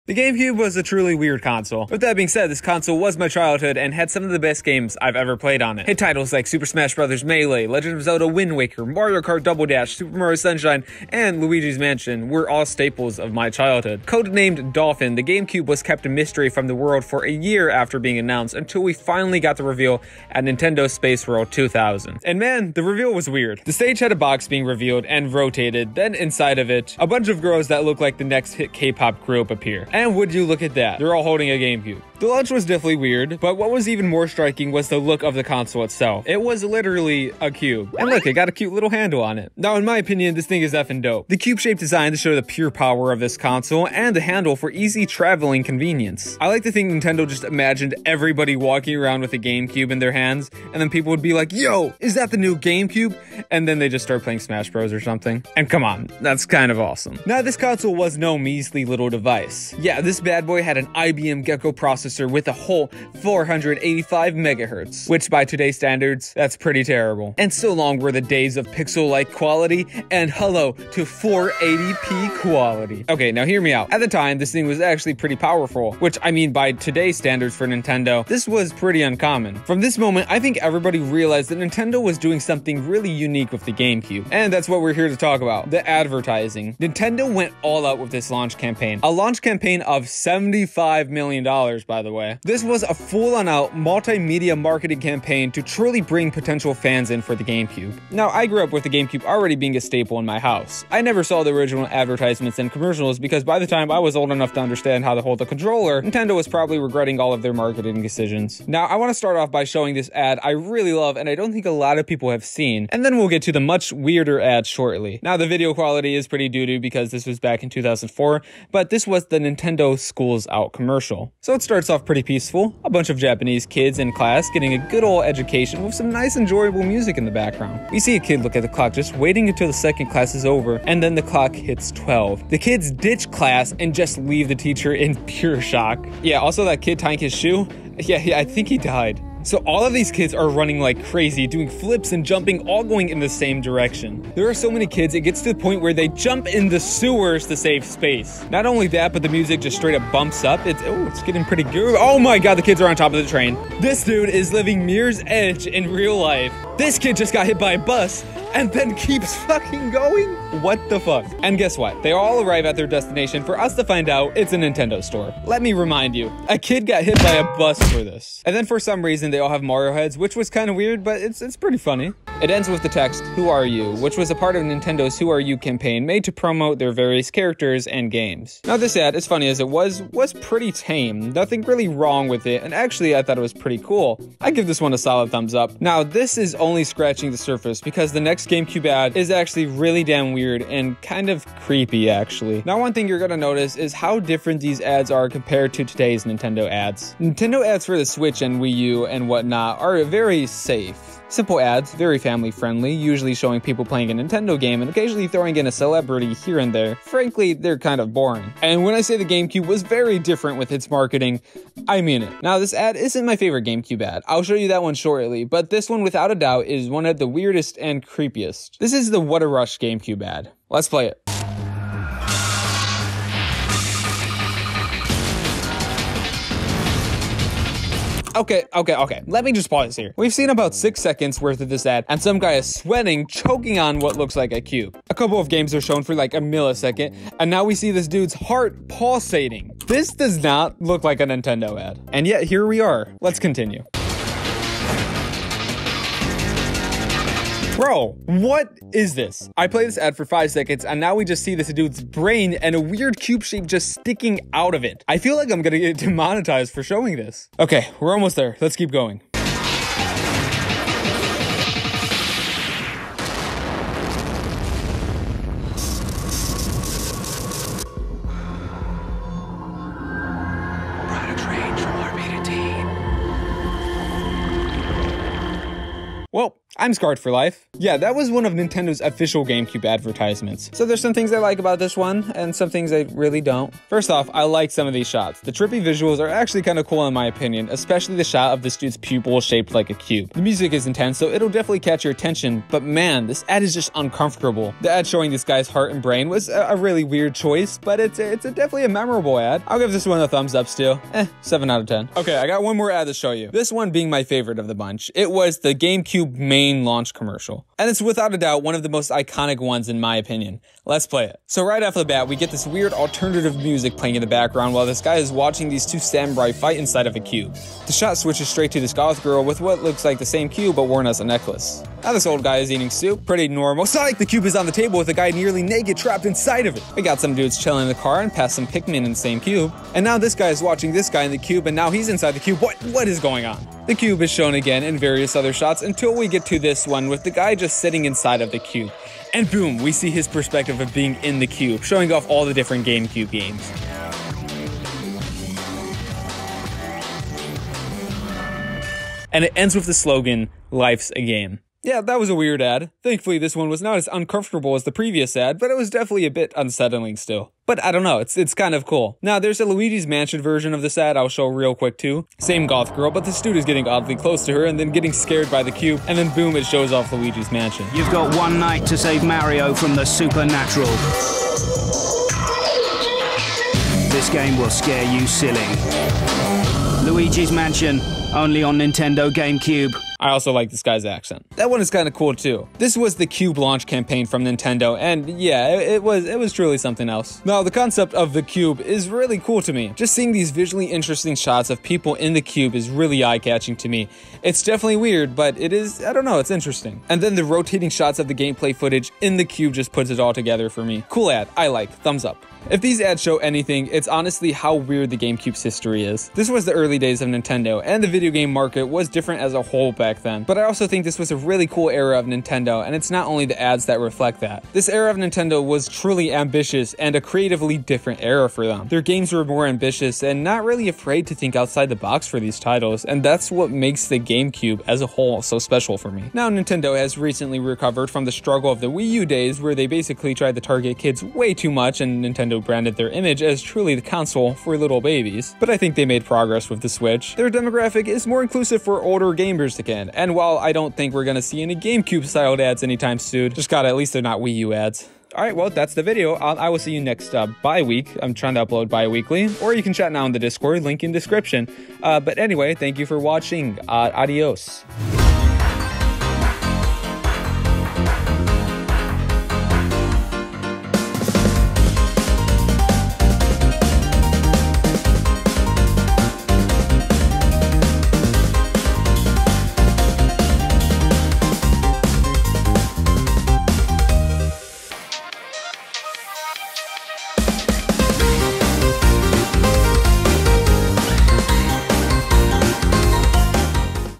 GameCubes Weird and Creepy Commericals
Let's take a look at some of the weirdest GameCube Ads from the early 2000's era. Definitely a weird time for Nintendo...